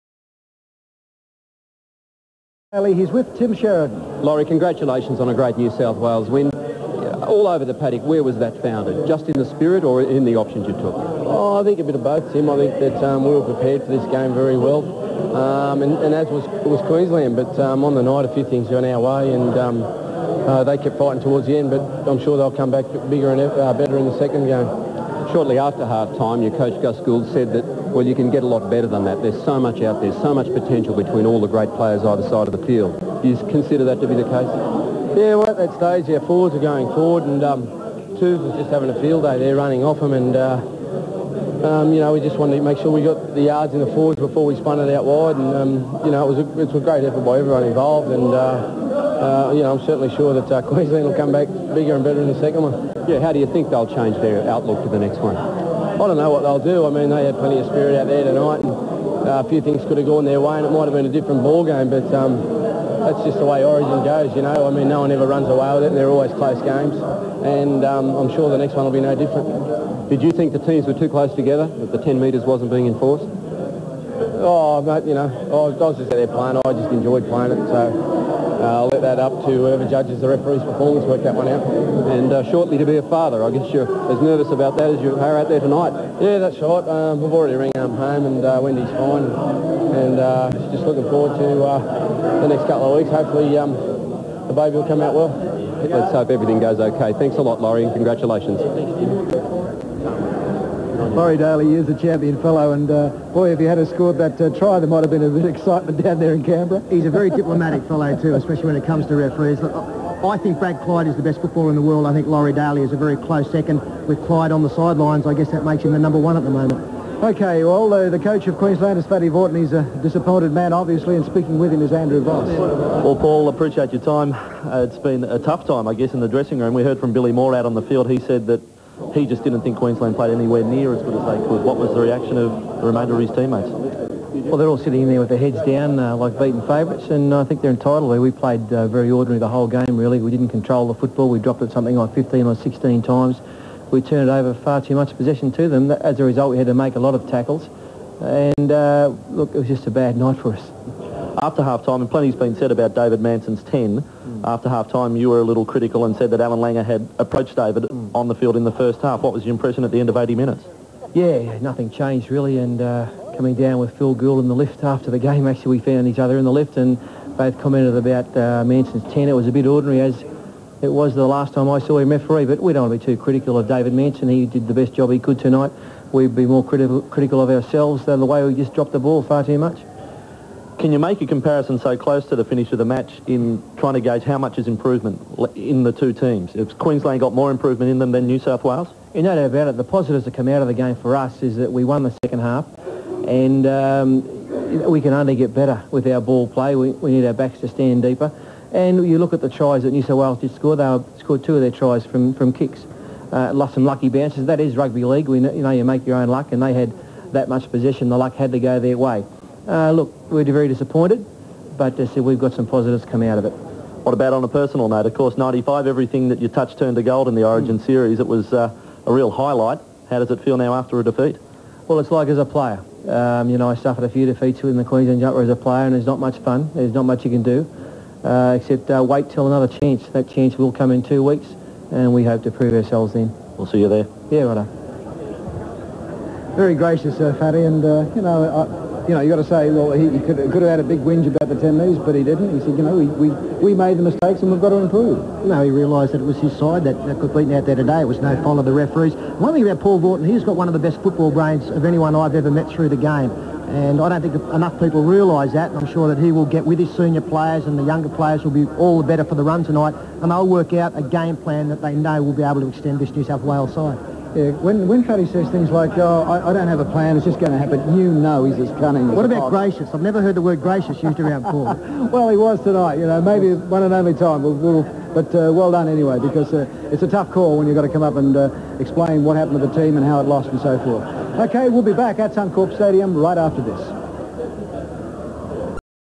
1996 ARL State Of Origin Match 1 Laurie Daley & Paul Vautin Post Match Interviews
1996 ARL State Of Origin Match 1 Laurie Daley & Paul Vautin Post Match Interviews. Sound & picture quality are not ideal due to transfer from VHS format.